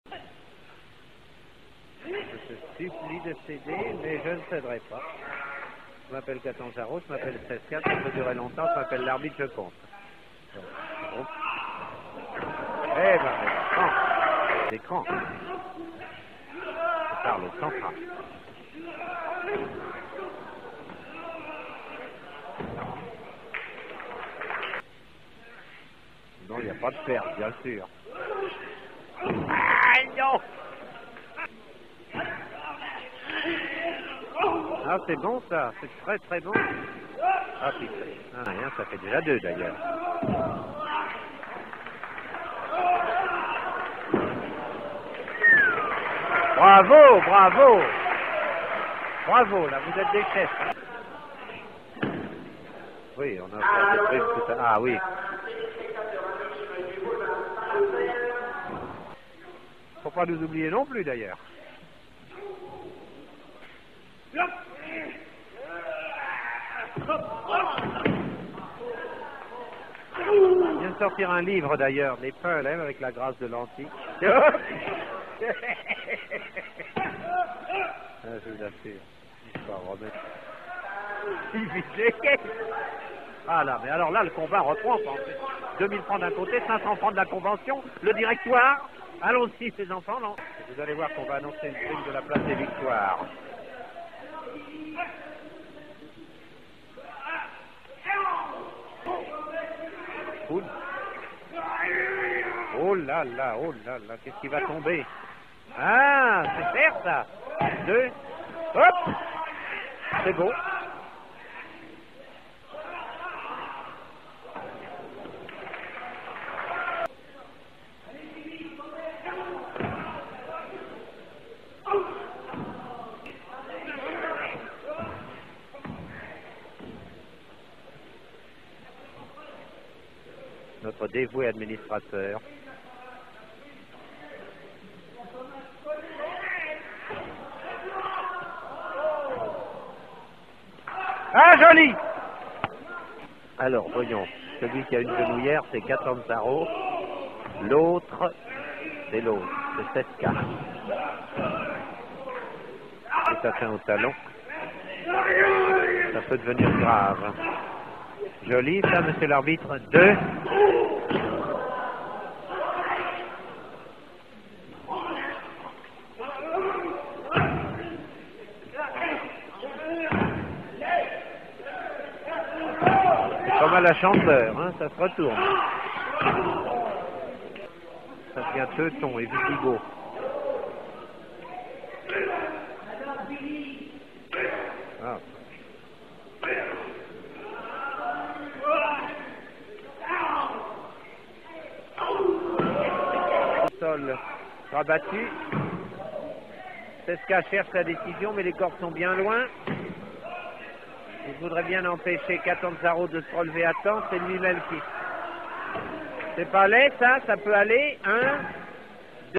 Insane exchanges from an old French wrestling match!
Highlights from the incredible Gilbert Cesca vs Billy Catanzaro match in 1957.